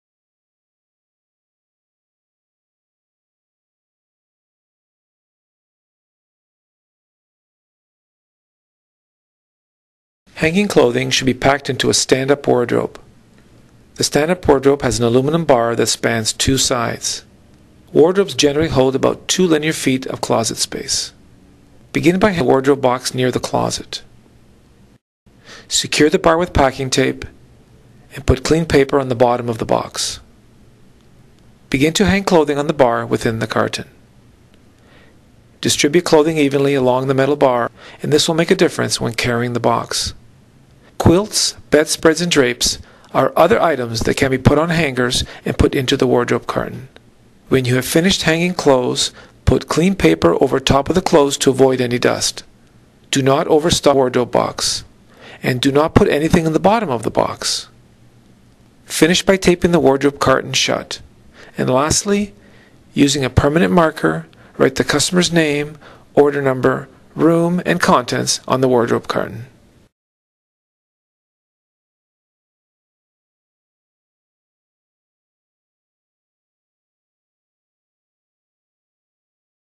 How to Professionally Pack Hanging Clothes in your Closet When you Move - Highland Moving & Storage
Some professional tips on how to properly and efficiently pack handing clothes and other clothing in your closet from Highland Moving & Storage.
for your FREE moving quote.

Moving With Pride since 1938